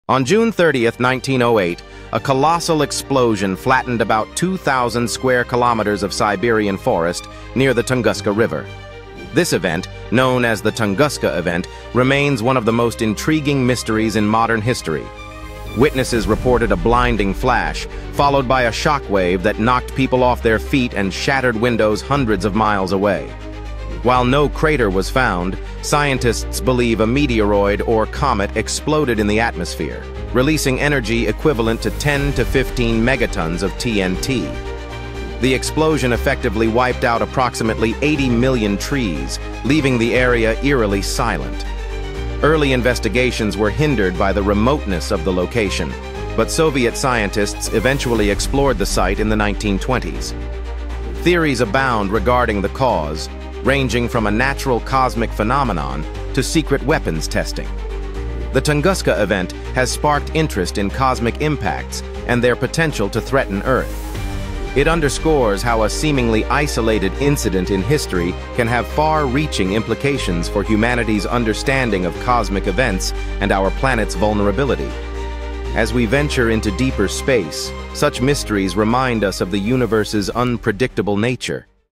The Tunguska Mystery: A Cosmic Explosion
Discover the enigmatic Tunguska Event of 1908 that rocked Siberia! What caused this massive explosion? Dive into one of history's greatest mysteries.